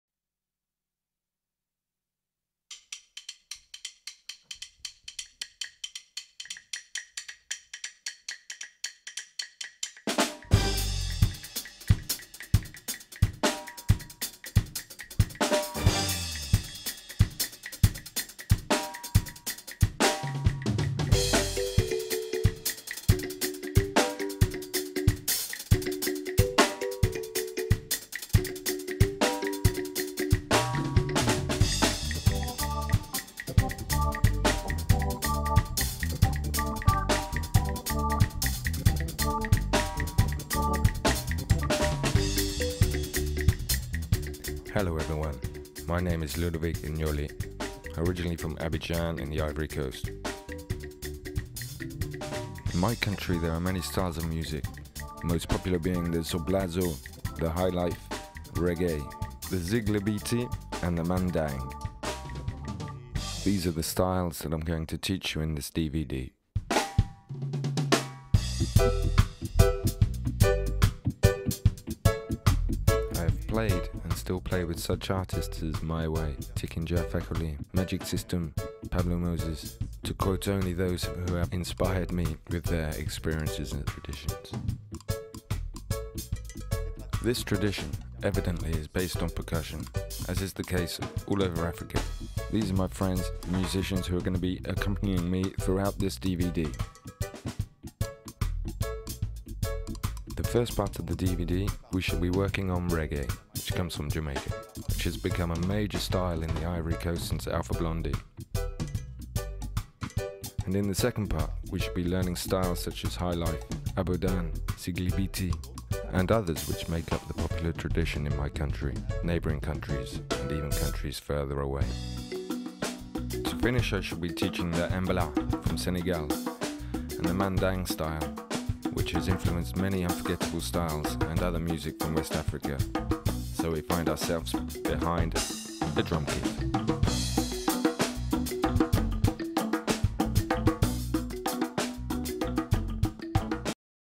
DVD AFRO-REGGAE DRUMMING by Ludo "Beelan Bull" N'HOLLE.
LA SORTIE DU DVD AFRO-REGGAE DRUMMING EST PREVUE POUR DEBUT 2015 ! UN PACK DE 2 DVD AFRO ET REGGAE A S'EN PROCURER DES LA MISE EN VENTE!
SOYEZ CONNECTEZ!

Production: Ludovic N'Holle, Guillaume Rossignol, Geoffrey Touboul, Yann Corbon.
Montage: Yann Corbon
Caméra: Eric Messan Amégan, Guillaume Rossignol
Calibrage couleur: Hugo Bachelet
Ingé son: Geoffrey Touboul
Doublage voix: Ben Wright
Traduction: Ben Wright, Shirley Melanie, Norbert Tranquille
Photo: Philippe Campos
Graphic Designer: Thierry N'Holle

Musiciens:
Ludovic N'Holle: Drum
Valery Assouan: Bass
Christian Mahileh: Clavier
Frantz Flereau et Nicolas Trancart: Percu